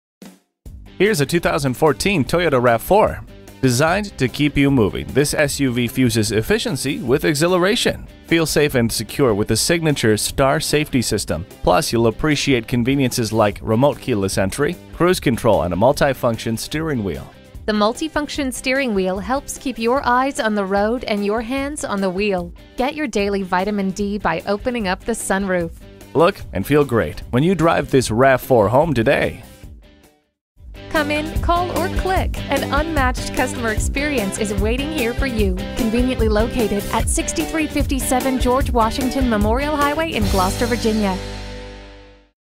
2014 Toyota RAV4 Gloucester VA Newport-News, VA #6077 - SOLD
Year: 2014
Make: Toyota
Model: RAV4
Trim: 6 Speed Shiftable Automatic
Engine: 4 Cylinder
Transmission: 6 Speed Shiftable Automatic
Color: Shoreline Blue
Interior: Black W/Fabric Seat Trim
Stock #: 6077

Address:
6357 George Washington Memorial HWY